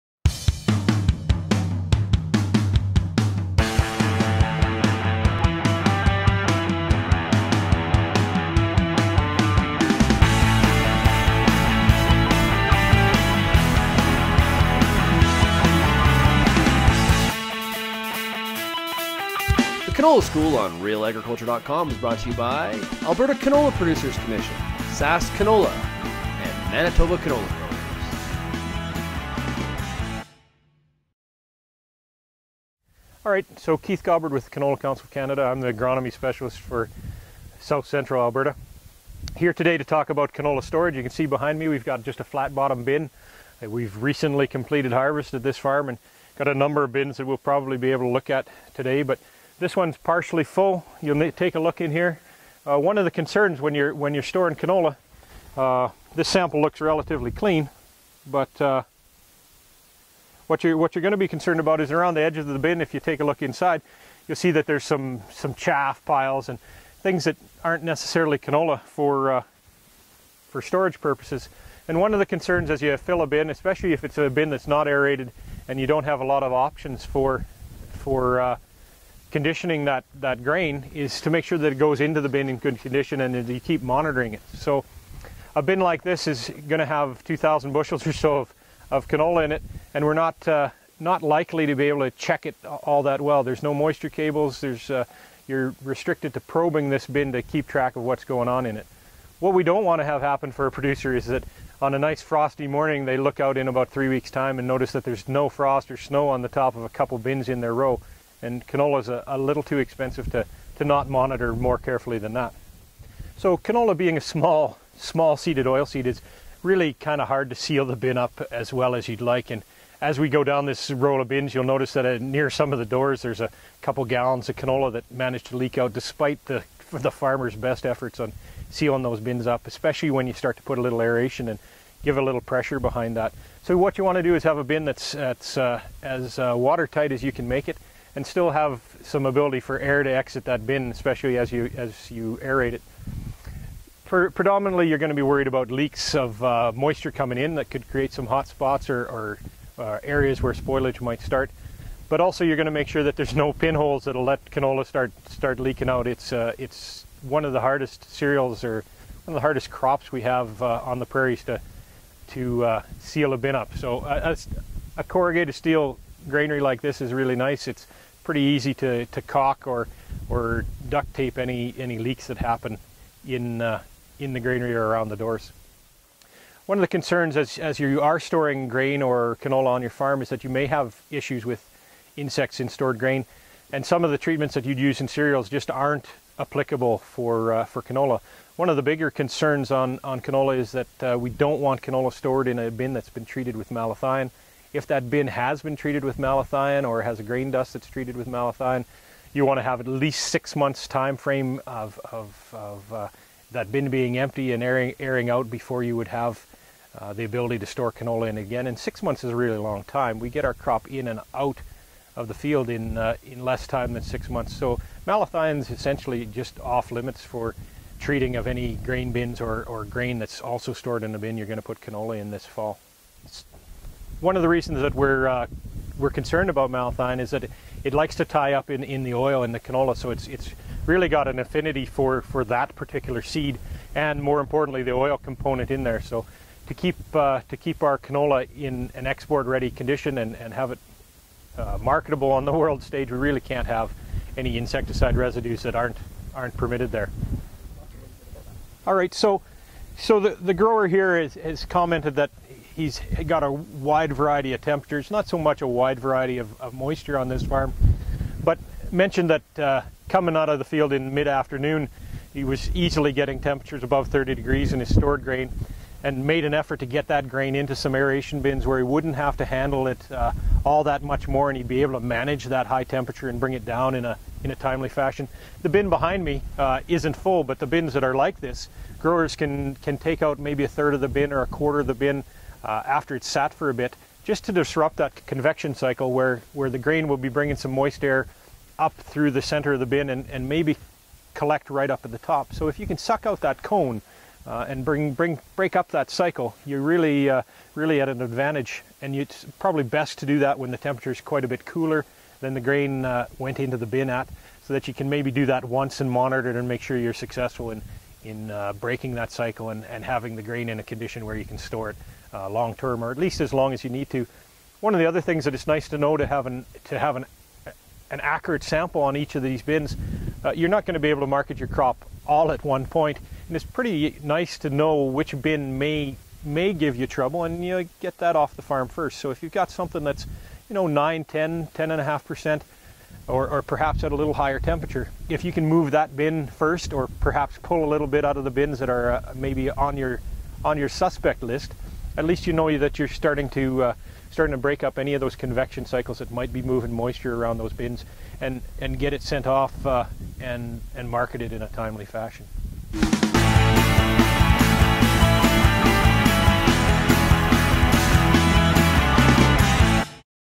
Canola School - Storing Canola
Keith Gabert of the Canola Council of Canada goes over some of the basics surrounding canola storage in this episode of the Canola School